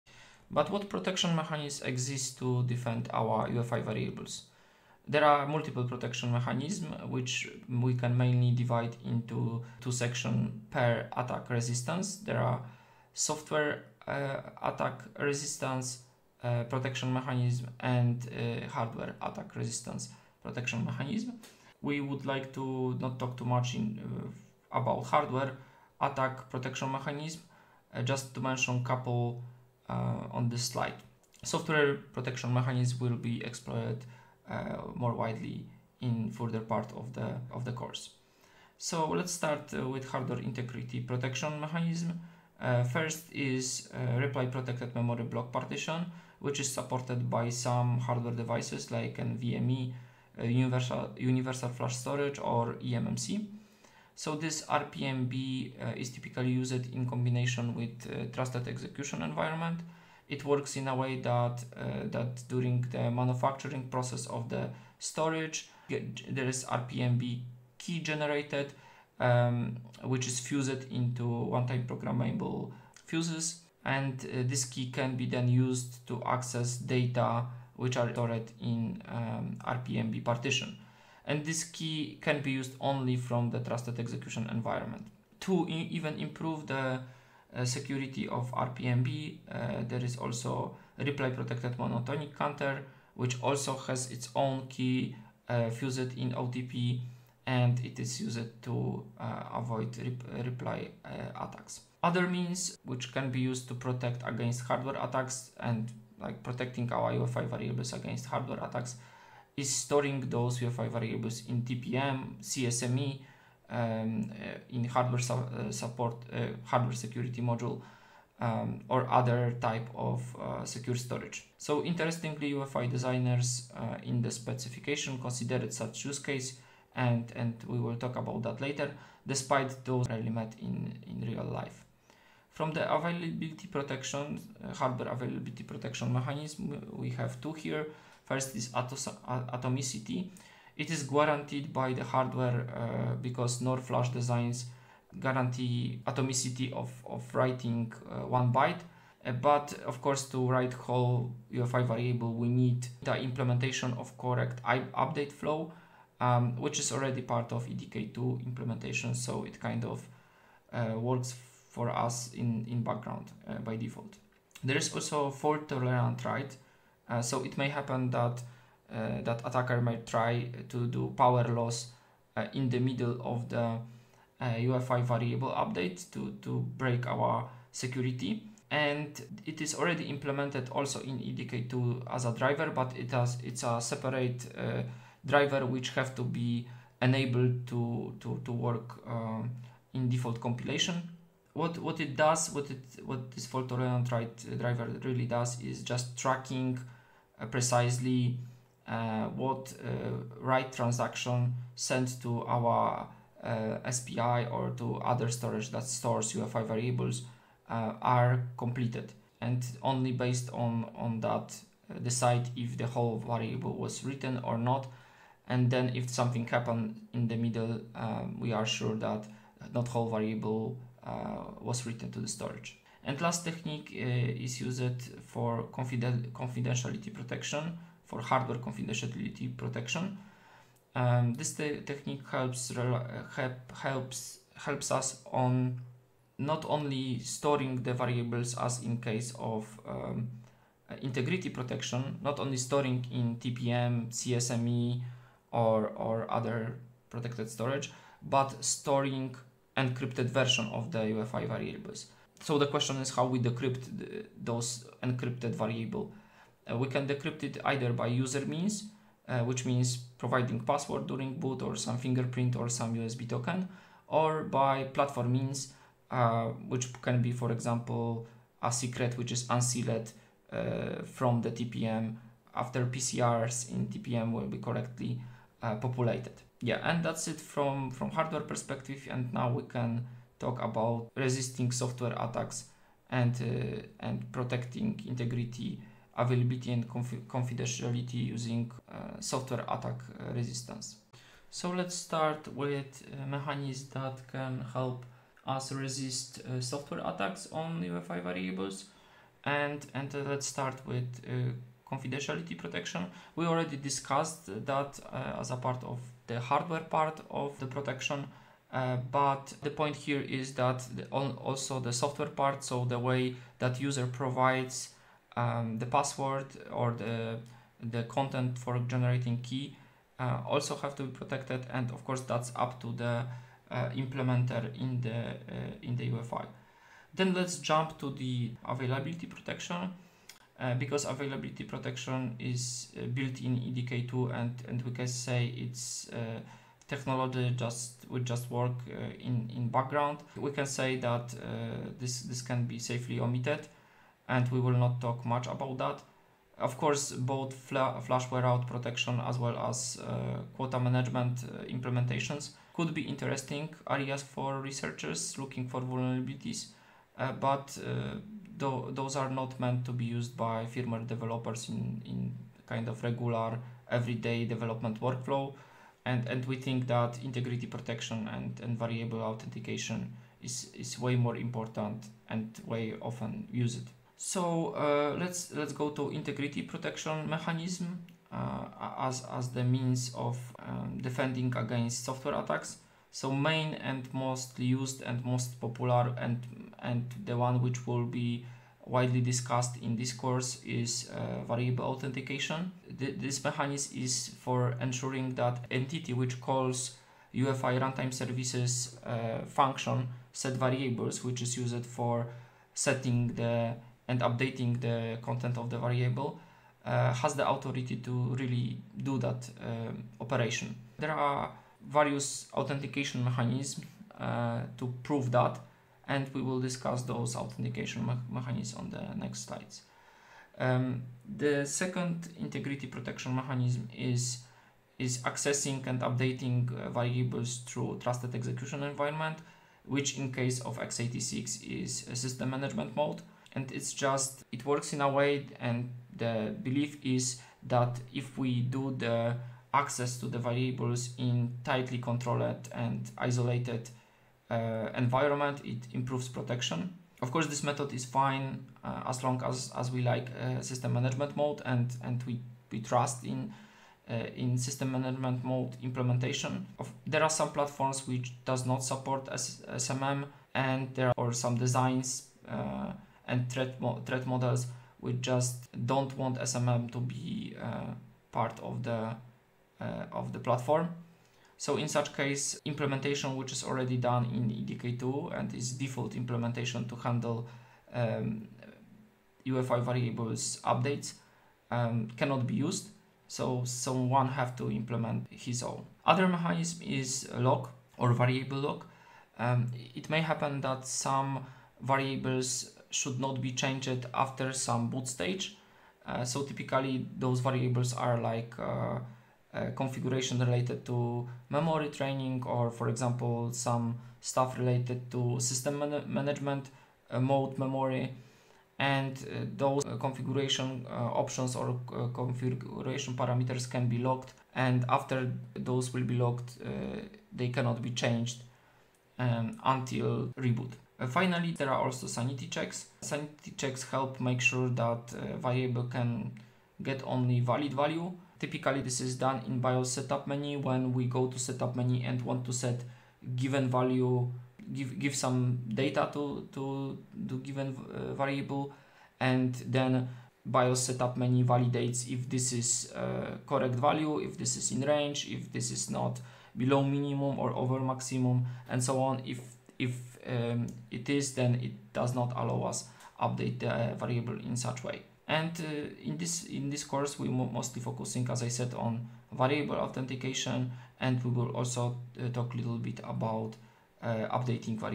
Arch4021: Introductory UEFI 05 02 UEFI variables protection mechanism
This course is an introductory guide to UEFI and PI specifications, touching upon firmware fundamentals and the boot process. It's tailored for newcomers and enthusiasts eager to gain basic insights into UEFI, its terminologies, and working with UEFI variables. If you're taking your initial steps in firmware development or looking for a foundational understanding of UEFI, this course is an excellent starting point.